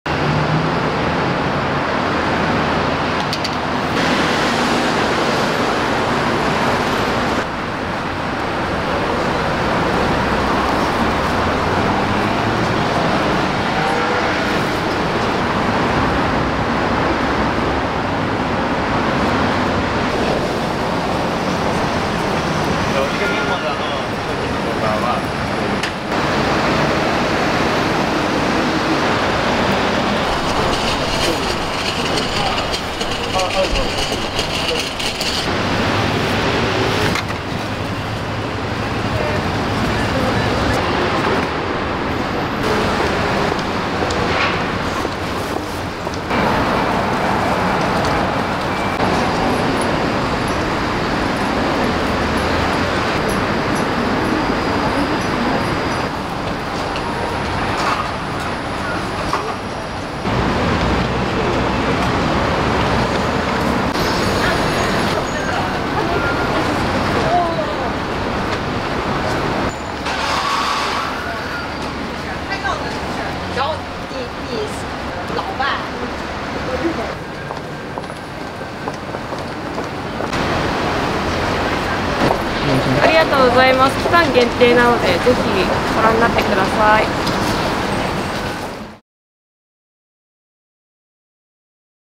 ミモザ Live Painting (by Lee Izumida)
2023/02/02
生活の木原宿表参道店のウィンドウに、絵描きのLee Izumidaさんがミモザのお花を咲かせてくれました。